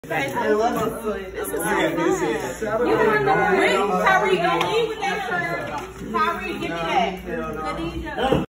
BTS EAB Photo Shoot
Eager & Blessed first photo shoot. The energy is the room was simply amazing.